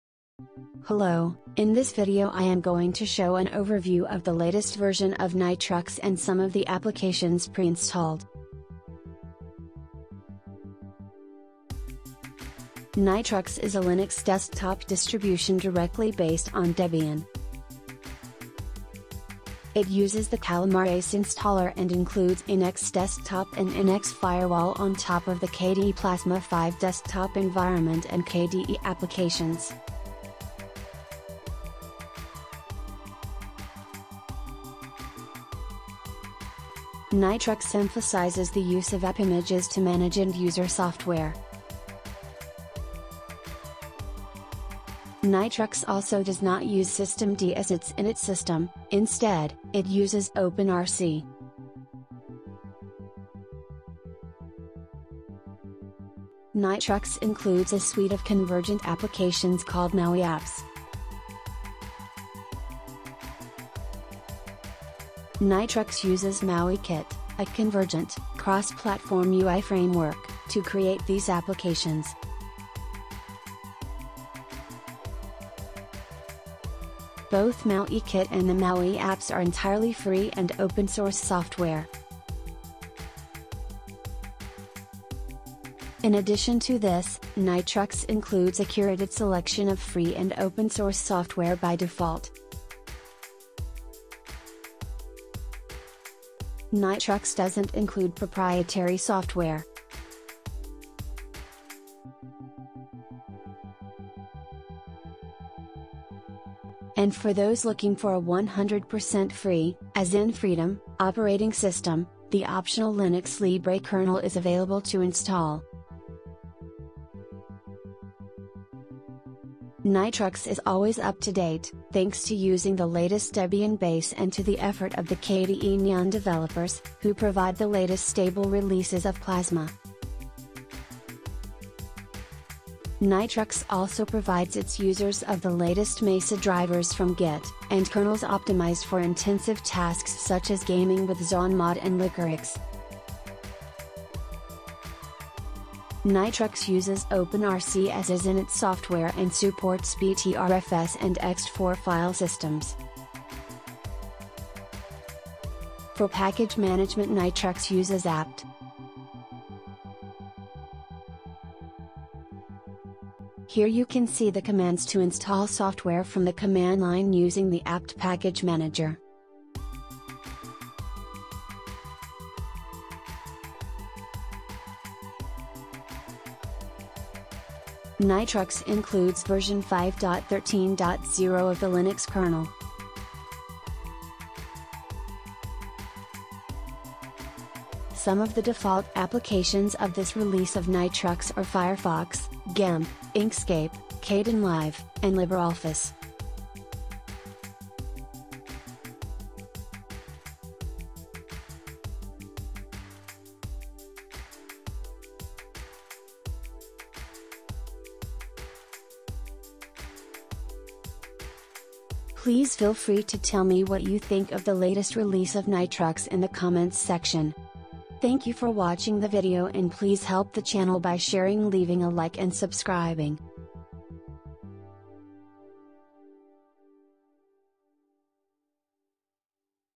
Linux overview | Nitrux 1.5.0
In this video, I am going to show an overview of Nitrux 1.5.0 and some of the applications pre-installed.

Nitrux is a Linux desktop distribution directly based on Debian. It uses the Calamares installer and includes NX Desktop and NX Firewall on top of the KDE Plasma 5 desktop environment and KDE Applications. Nitrux emphasizes the use of AppImages to manage end-user software. Nitrux also does not use systemD as its init system; instead, it uses OpenRC.